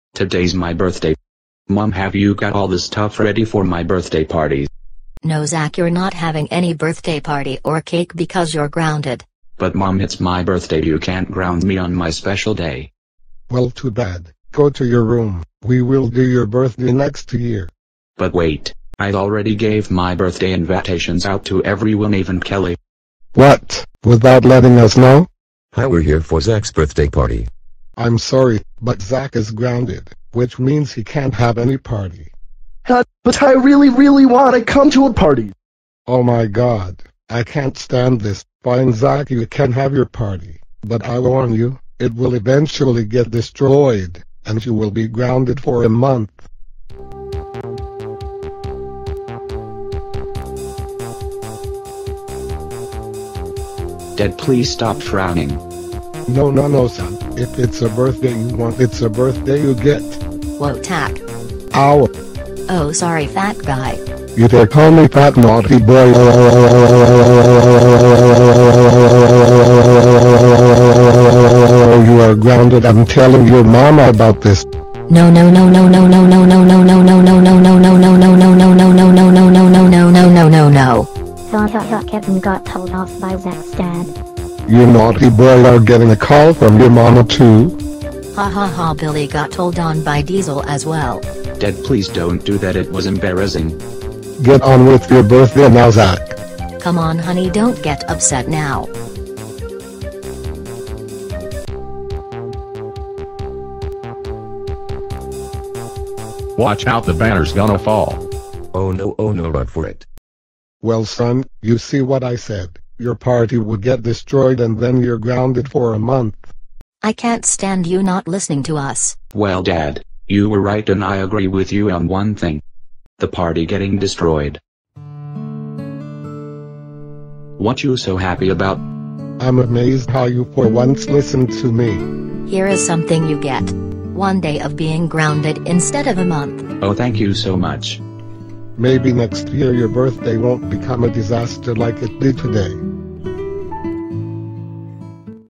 Zack's destroyed birthday
This video was originally created by the original Zack creator! Since this video is GCU-canon, it takes place on February 11, 2012.

ORIGINAL DESCRIPTION: It's Zacks 15th birthday and it went all bad until the end it got good.